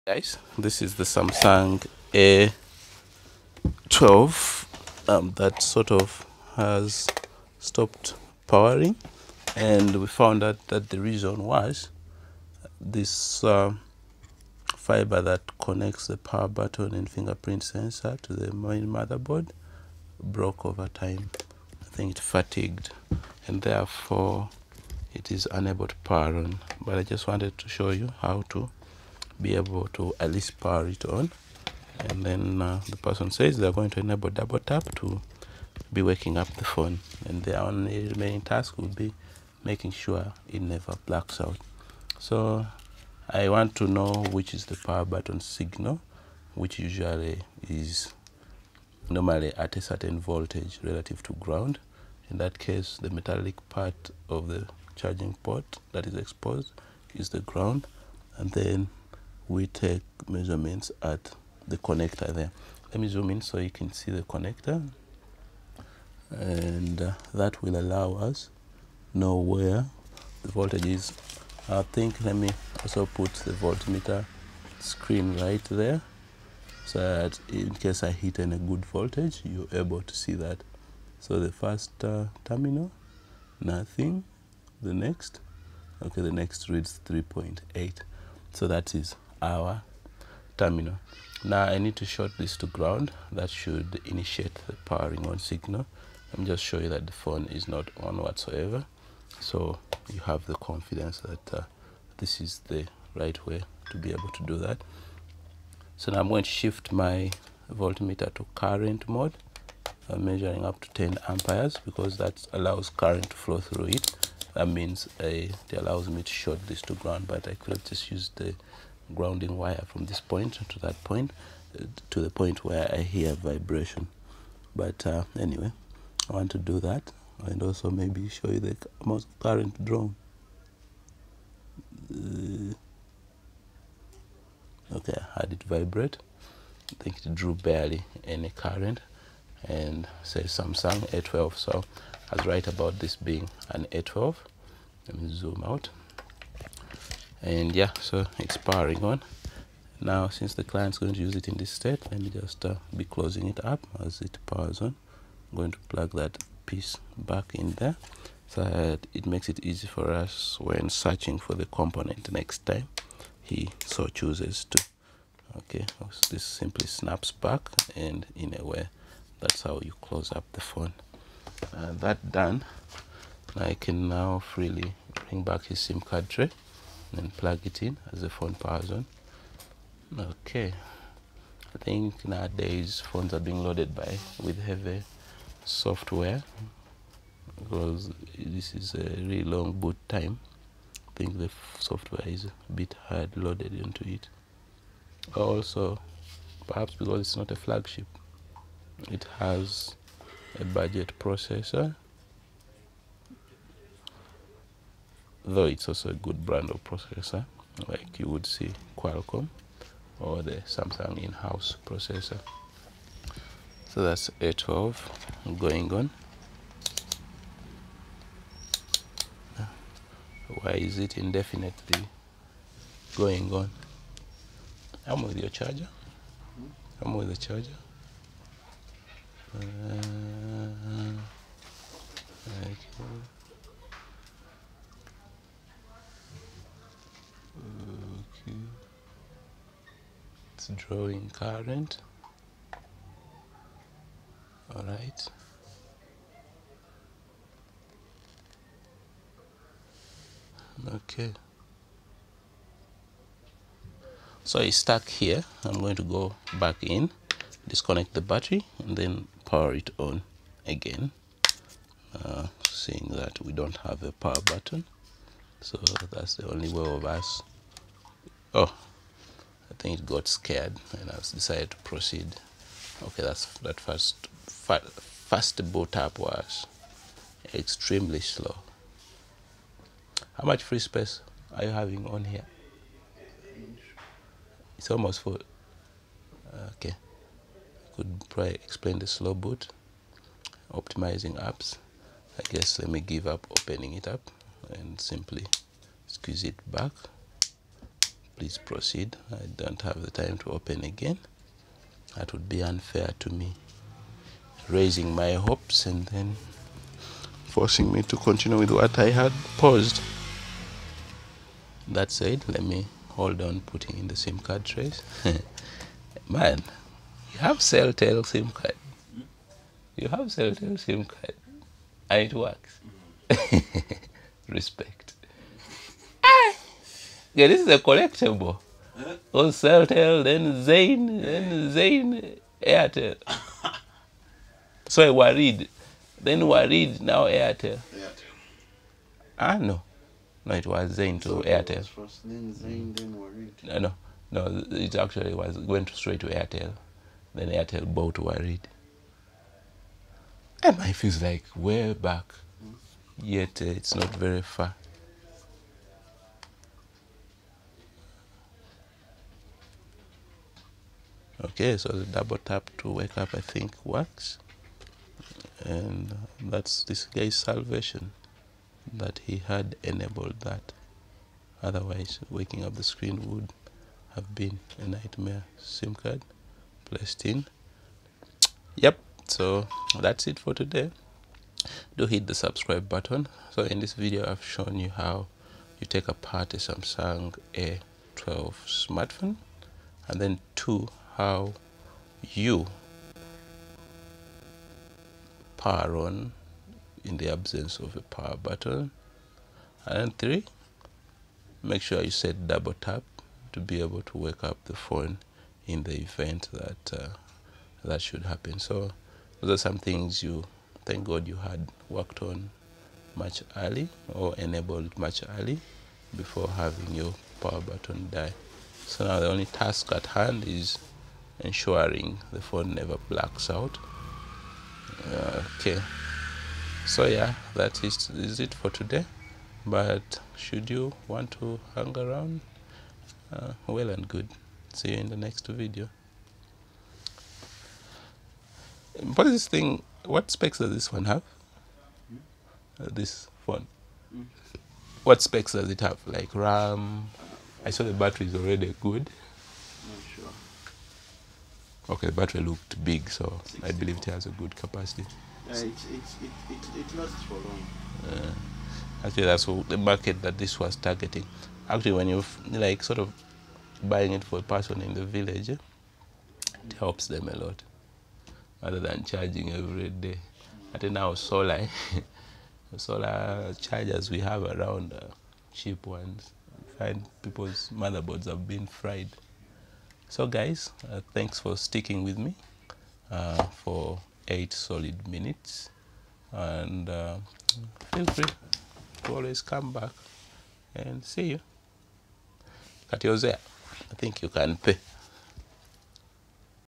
How to boot a Samsung Galaxy A12 smartphone with dead power button
Force it to turn on:

A Repair Video for Fixing, Repairing, Samsung Galaxy A12 Smart phone oh mobile phone that has a dead power button because the fibre tore.